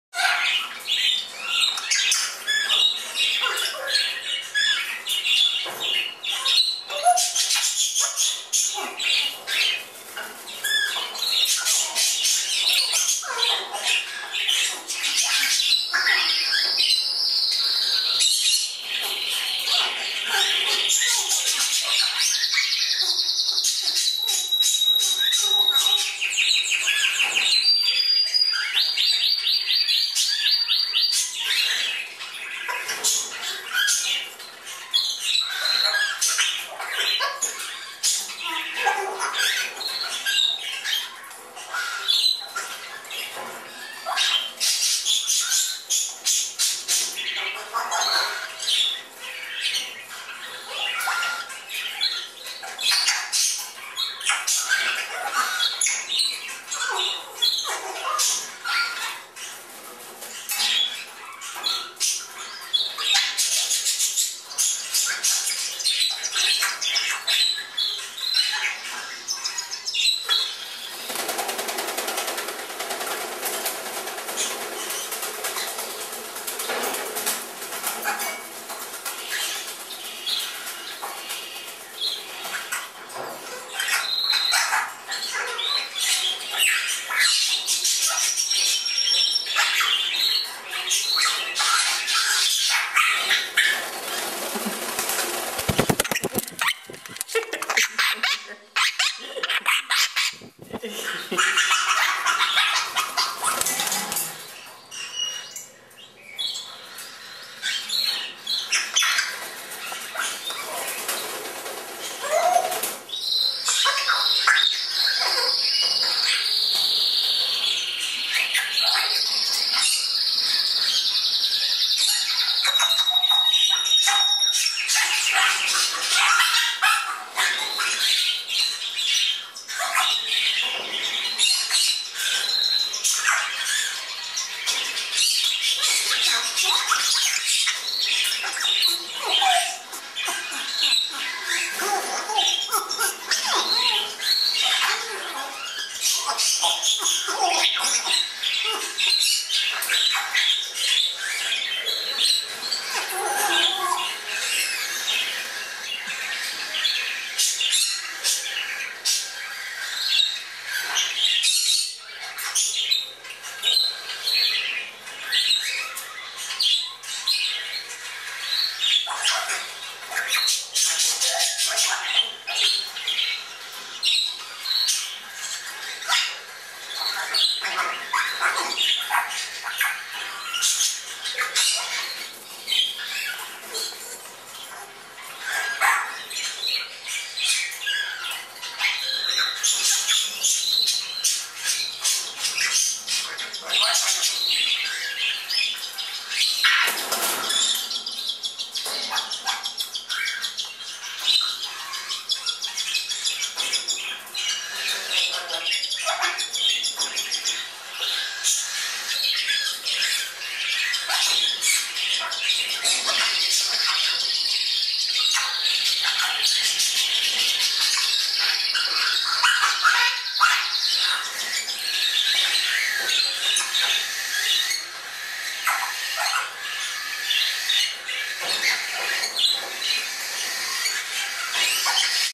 African Grey Parrots flying in Dallas Texas
These are fledging Congo & Timneh African Grey Parrots at our bird shop just north of Dallas. WE have everything you need for your africangrayparrot be sure and check out our